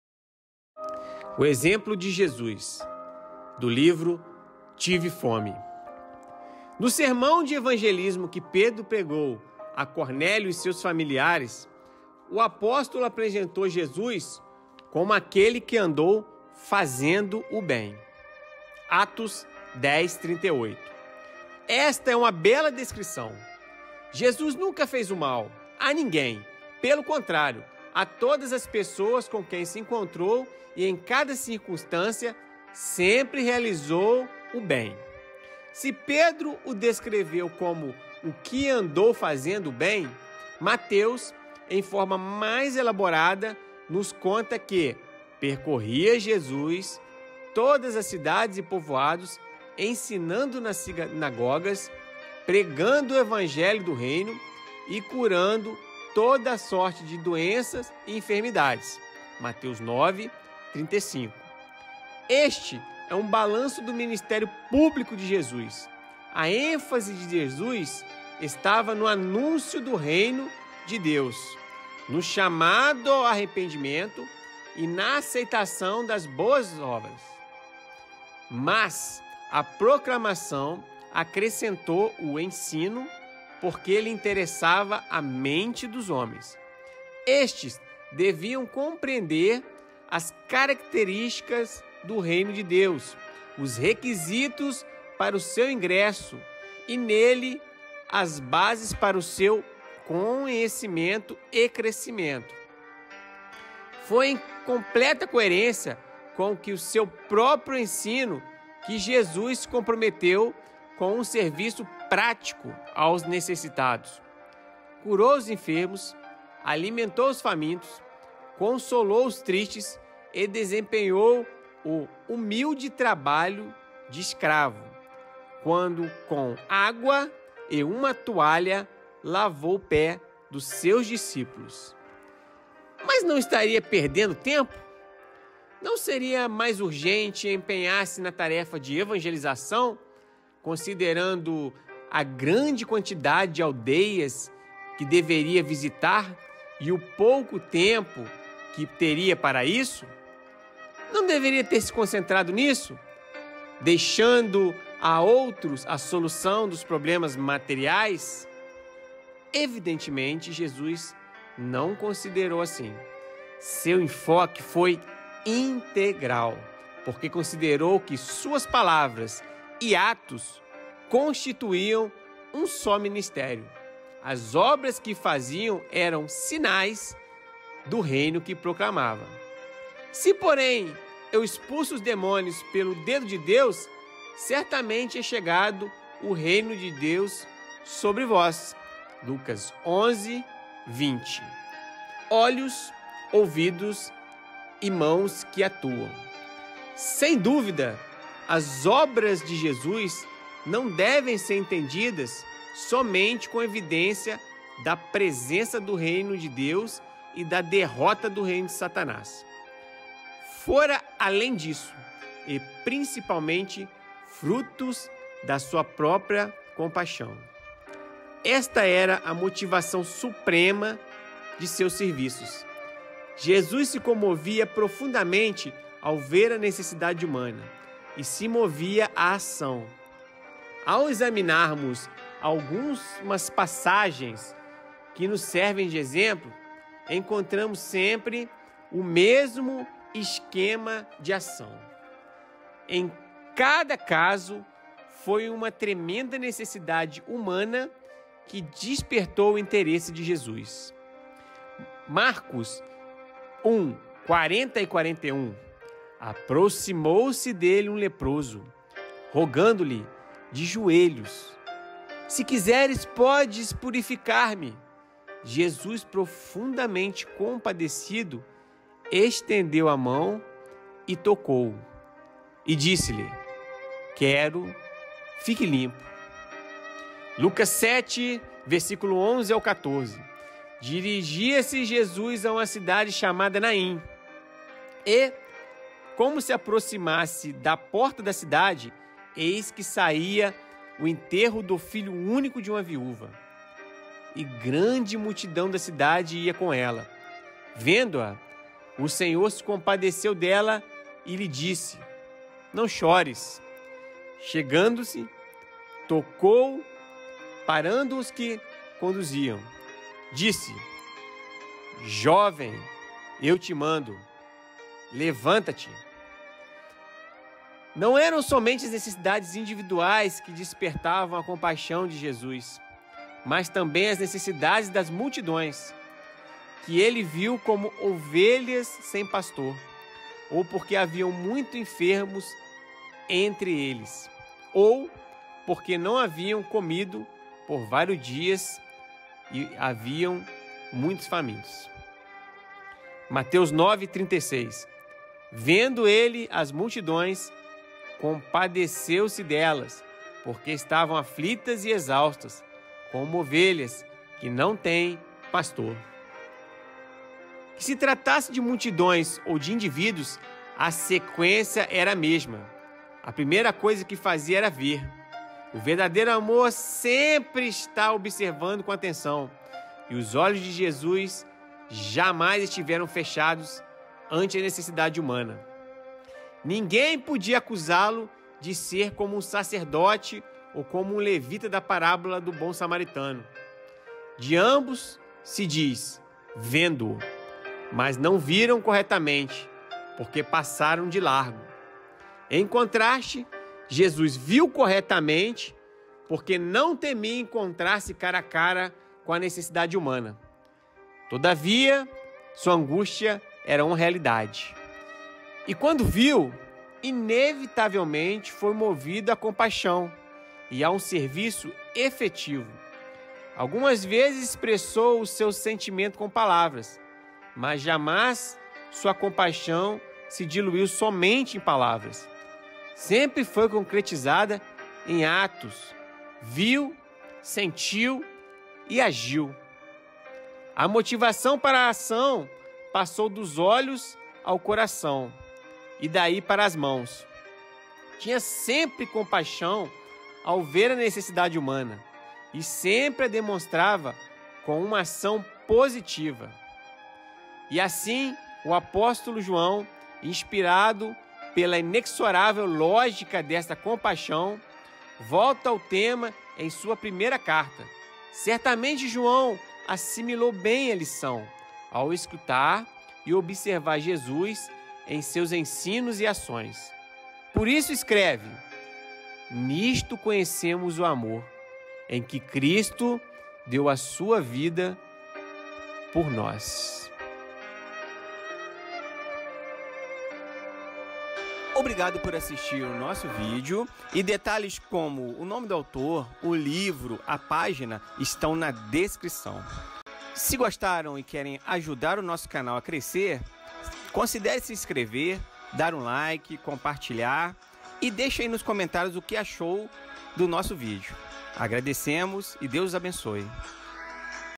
Jesus nosso exemplo
Trecho retirado do livro Tive Fome, focado no exemplo de Jesus, excelente trecho para sua reflexão.